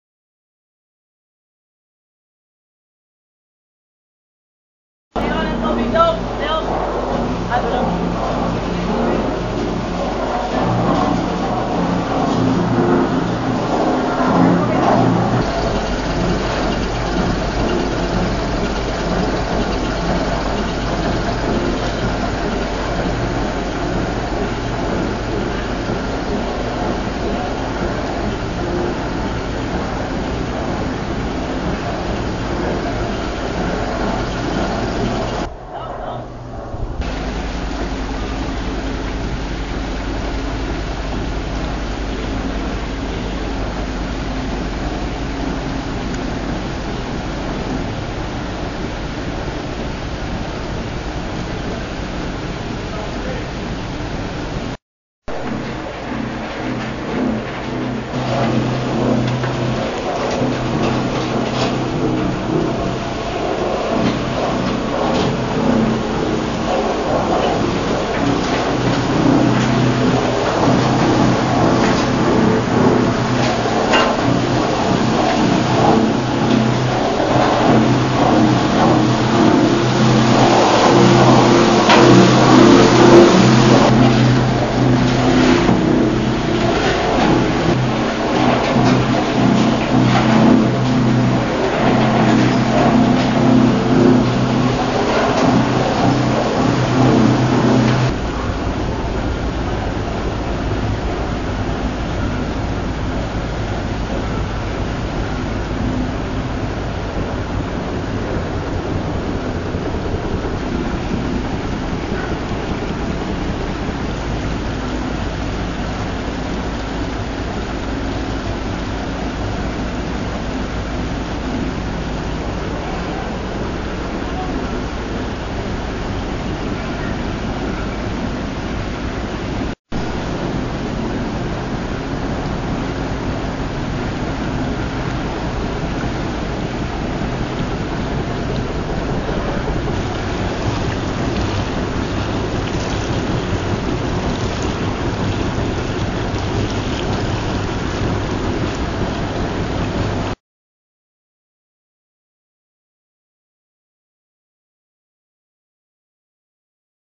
The small scaled site of alluvial gold in Philippines
The site is located in Philippines. The primary machines are rotary scrubber, jig, shaking table, amalgamation tank, etc.